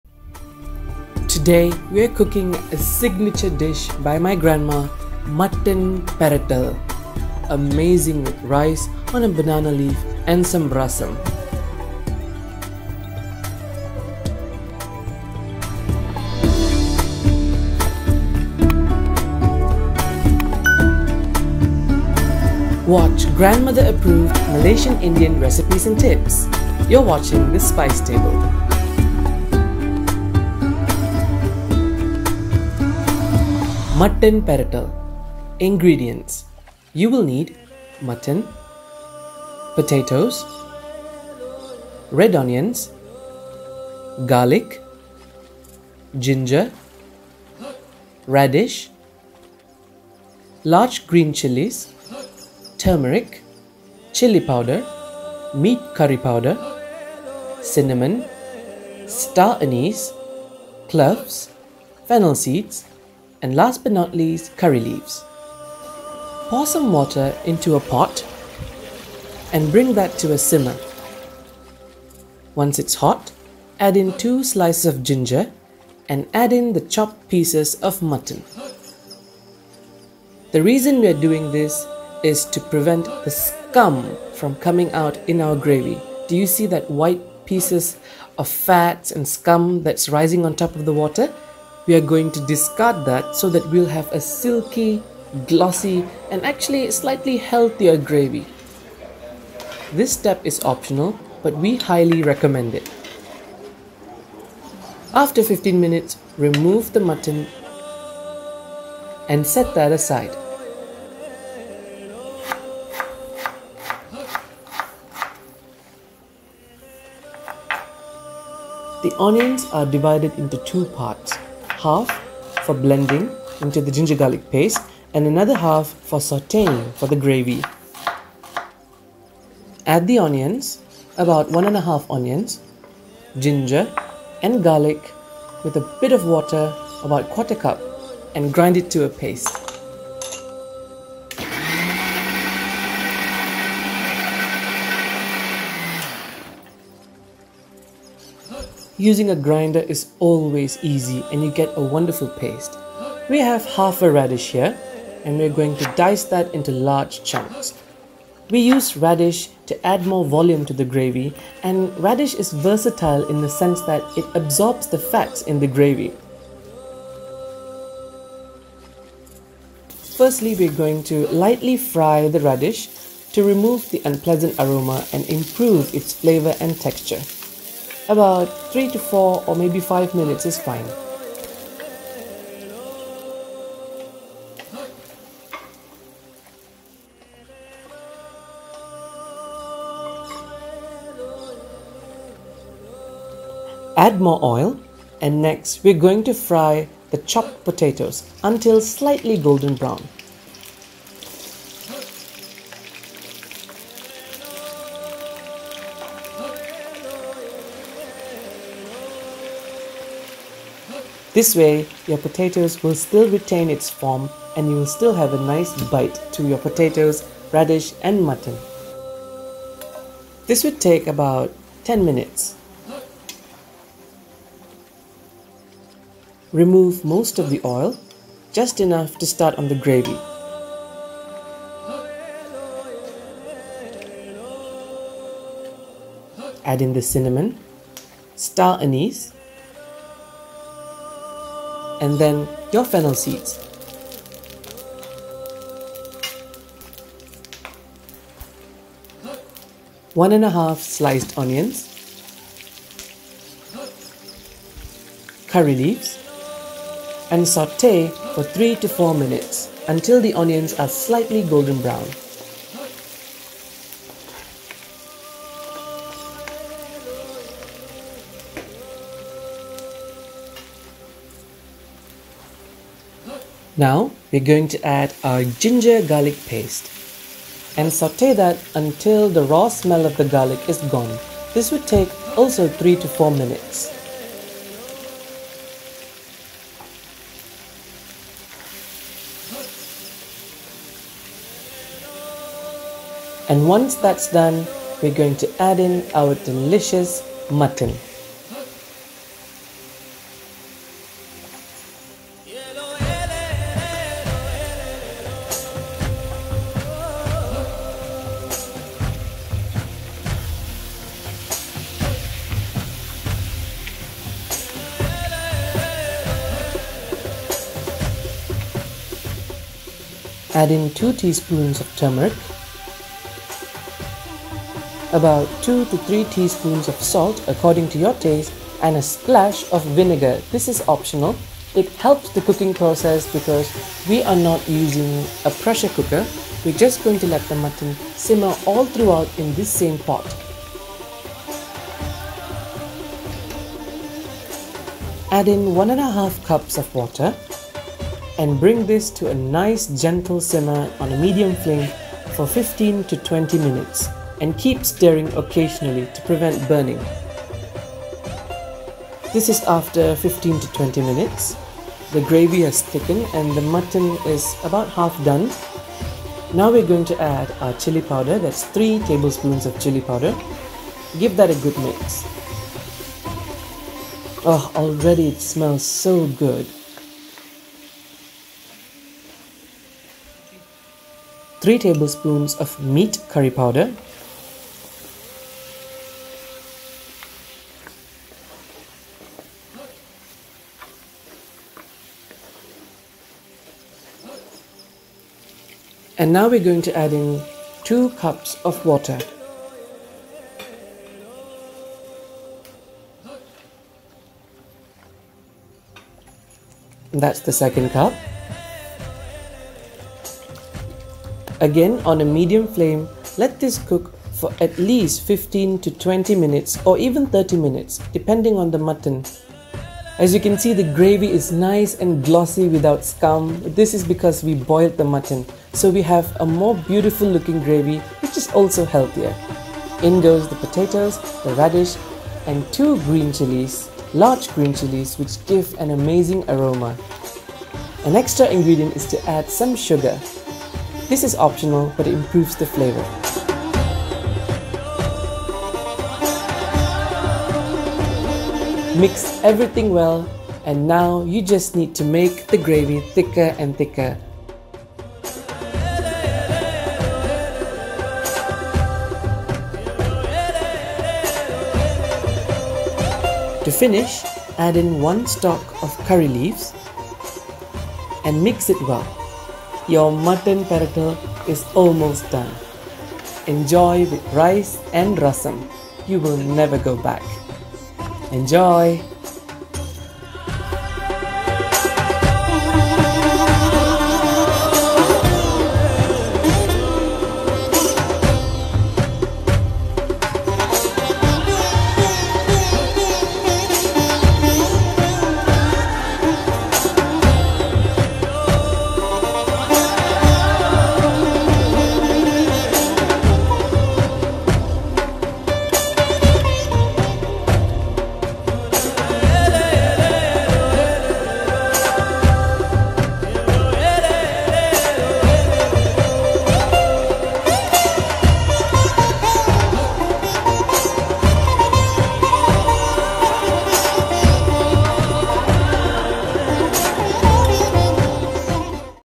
Grandmother's Mutton Peratal: South Indian Spicy Mutton Gravy
Spice Table Season 2: Episode 20: Grandmother's Mutton Peratal

Today, we are going to learn the ultimate classic of any Malaysian Indian household, Mutton Peratal. This is a South Indian spicy mutton gravy dish. Prepared with tender chunks of mutton, radish and potatoes stewed in a thick spicy gravy, this is a delicious main dish served with rice and rasam. An amazing recipe to try out, don't miss this one!

(INGREDIENTS)
1kg mutton
2 potatoes
3 red onions
12 cloves of garlic
4 inches ginger
1/2 radish
2 tsp turmeric
3 tbsp red chilli powder
3 tbsp meat curry powder
2 cinnamon sticks
2 star anise
4 cloves
2 tsp fennel seeds
2 stalks curry leaves
salt
oil
water

(OPTIONAL)
1 tbsp vinegar
1 tbsp sugar